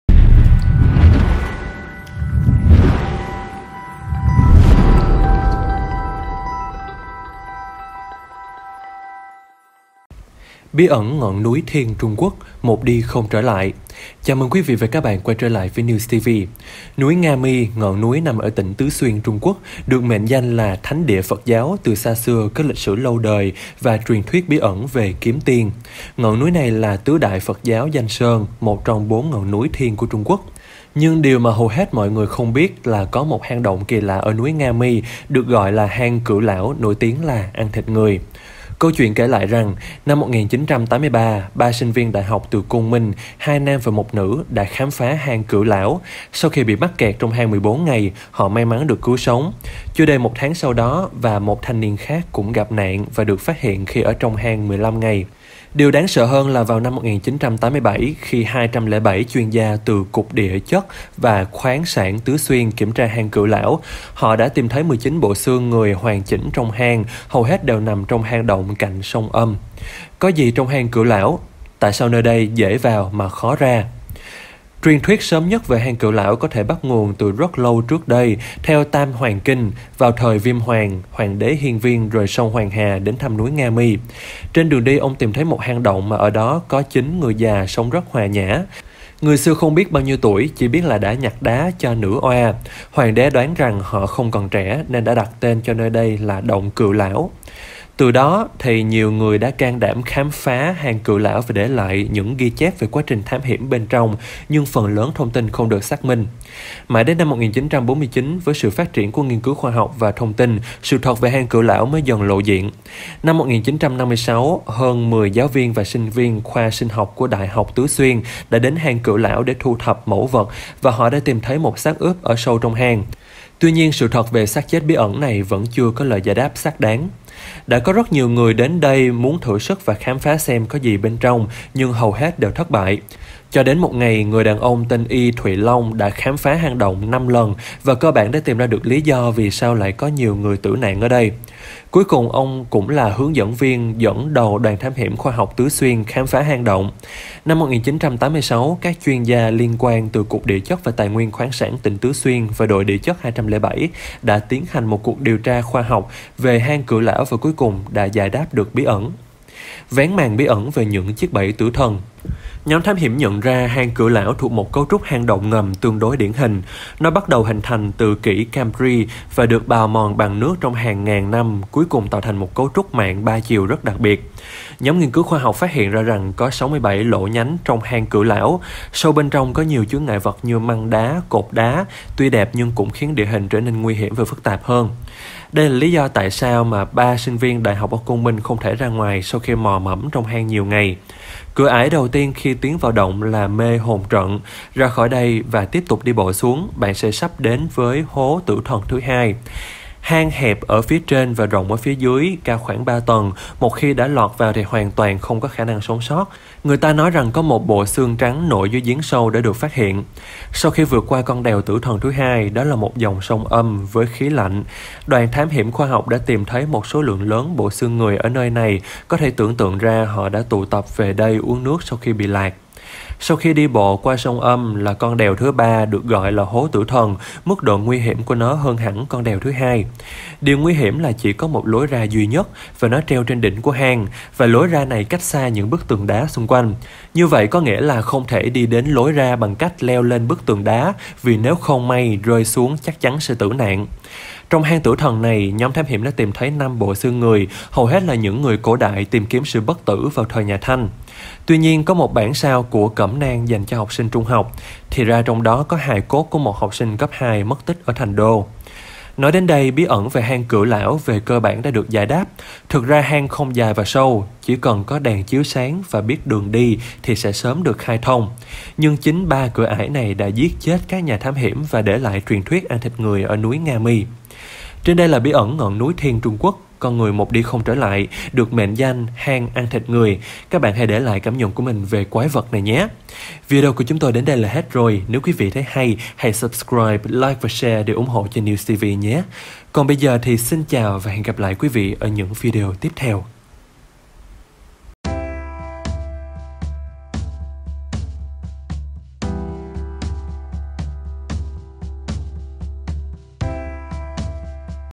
Bí ẩn 'ngọn núi thiêng Trung Quốc' - Một đi không trở lại
[News TV] - Núi Nga Mi (ngọn núi nằm ở tỉnh Tứ Xuyên, Trung Quốc) được mệnh danh là 'thánh địa Phật giáo' từ xa xưa có lịch sử lâu đời và truyền thuyết bí ẩn về "Kiếm Tiên".